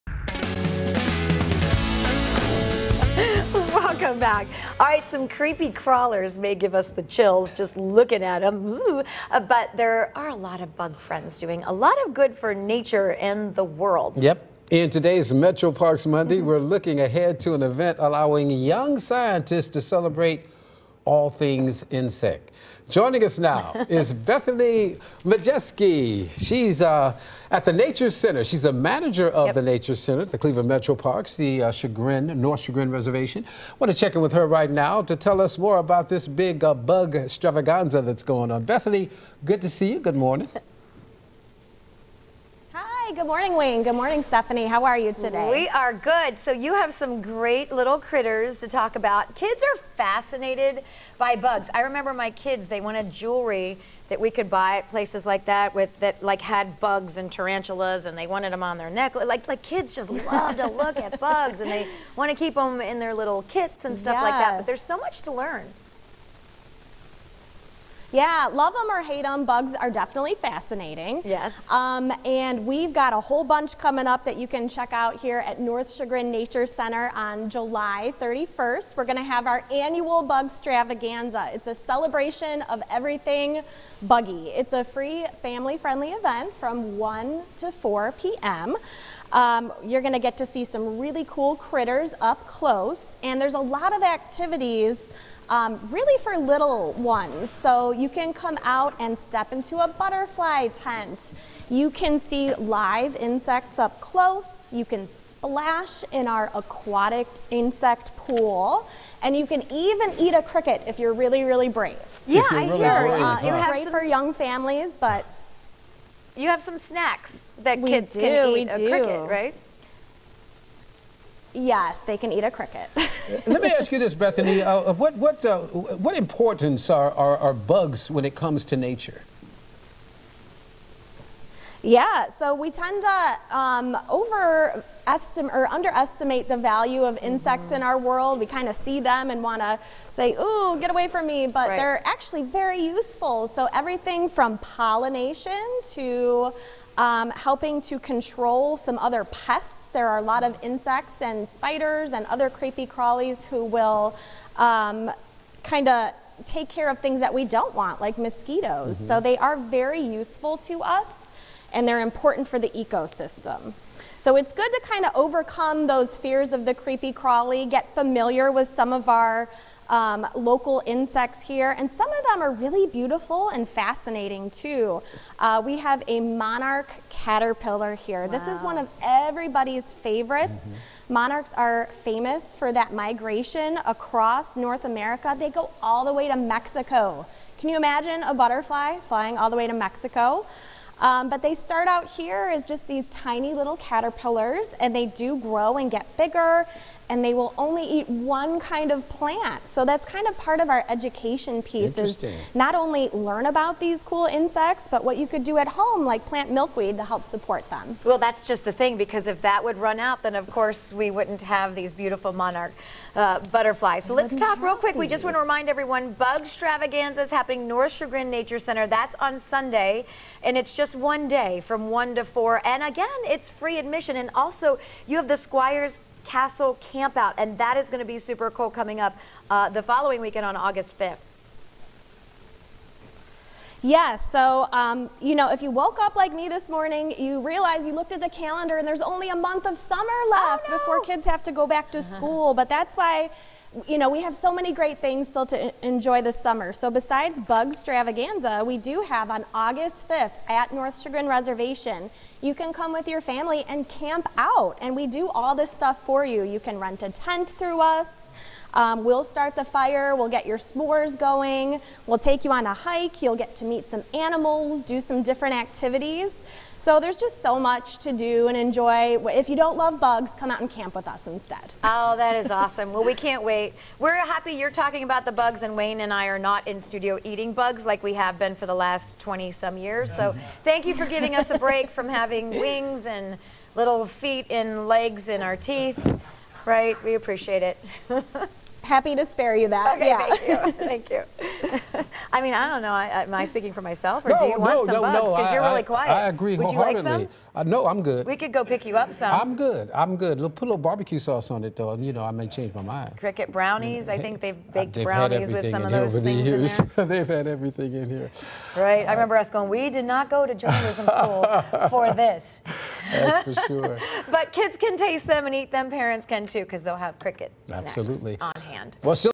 Celebrating Bugs & a Castle Campout -- Cleveland Metroparks summer fun
The Cleveland Metroparks has many fun summer events happening all across Northeast Ohio. Bug-Stravagnza happens July 31st at the North Chagrin Nature Center and a Squire's Castle Campout is offered August 5th. Fox 8's Stefani Schaefer and Wayne Dawson get all details.